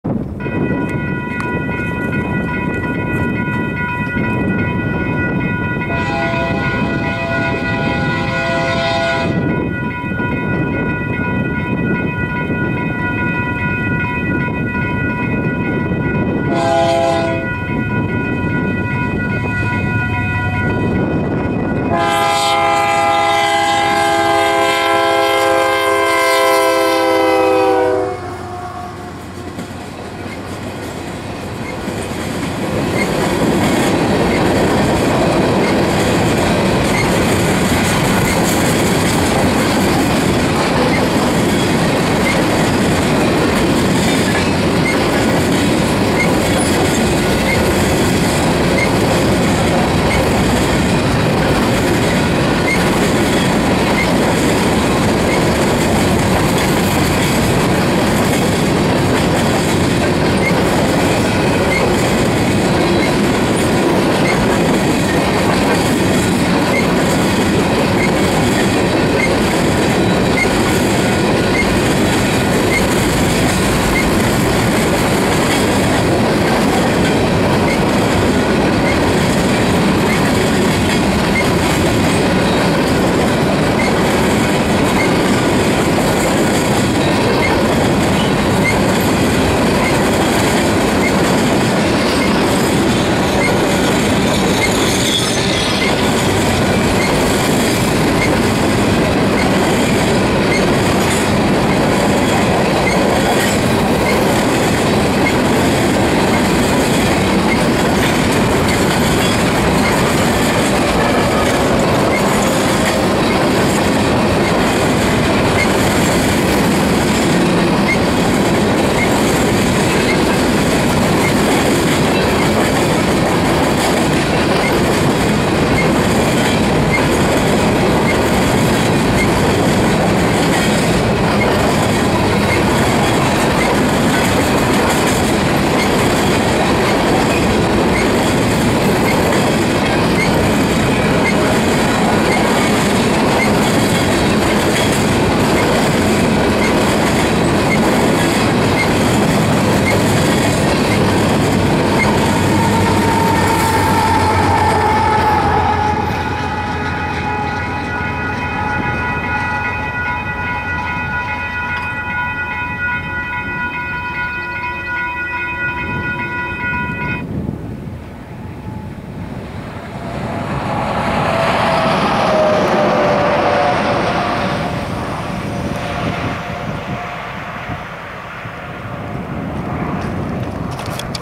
eastbound Kansas Grain Train on the GSR 05/31/2014 # 9
eastbound Kansas grain train at MP 294.6
UP 1st Generation C44ACCTE # 5616, UP SD70M # 4849, DPU SD70M # 5106, UP 1st Generation C44ACCTE # 5736